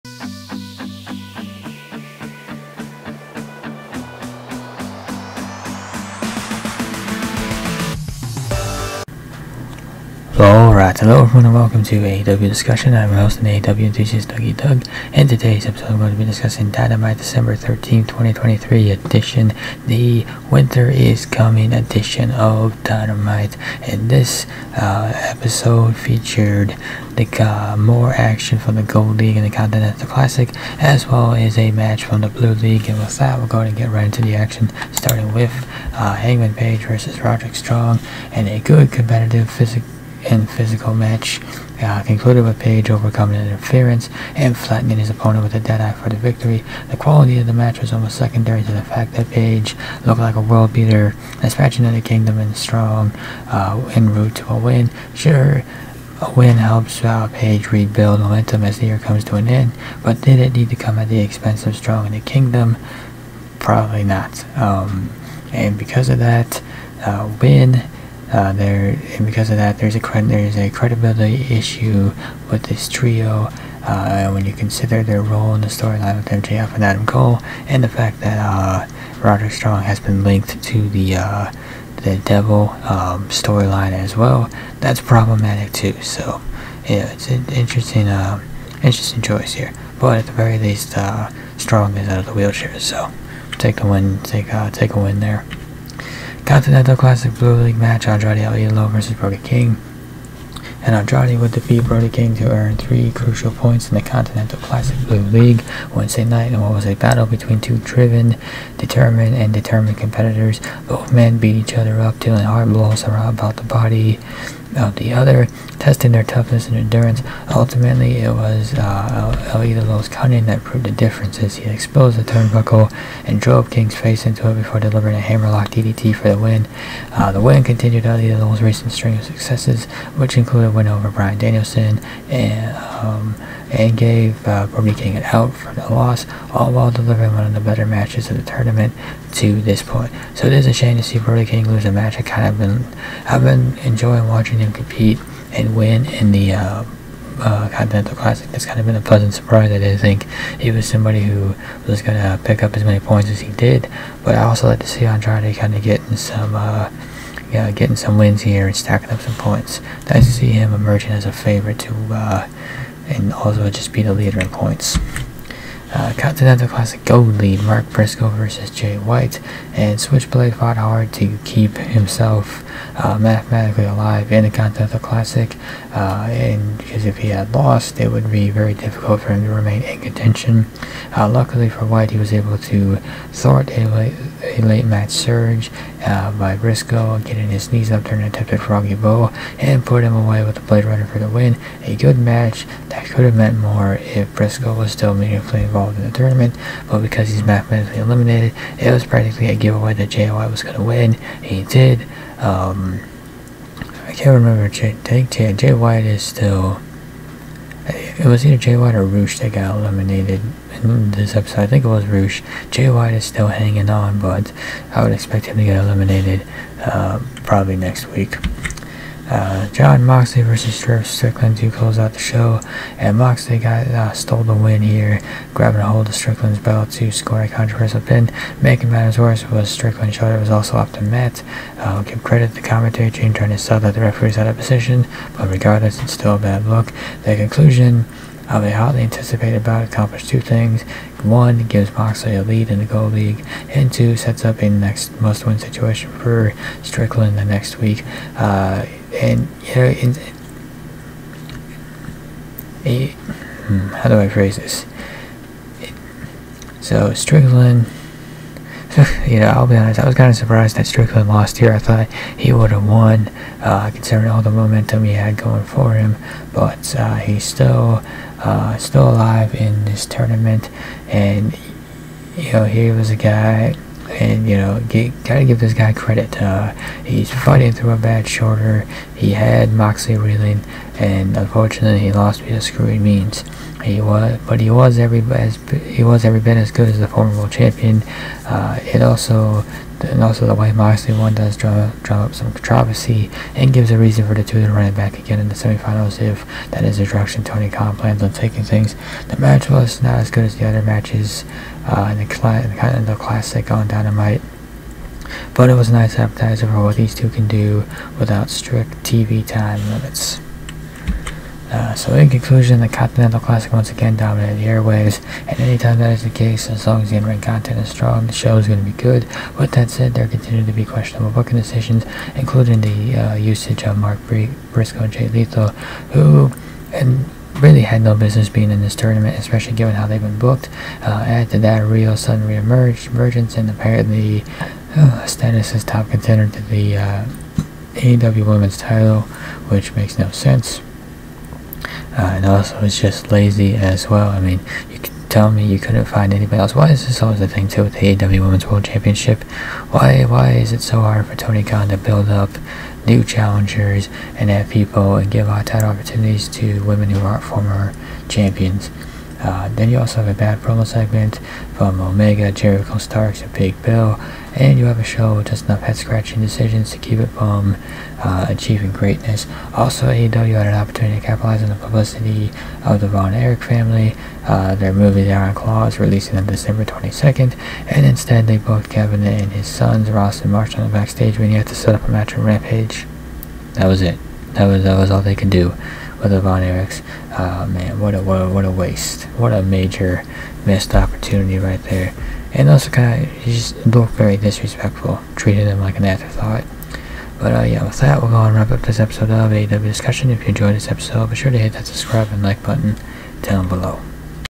AEW discussion episode 28- Dynamite (12/13/23) Winter is Coming
Hello and welcome to AEW discussion. In todays episode I discuss AEW Dynamite.
Shoutaht to @socialdesignzyt (Twitter) for creating my logo.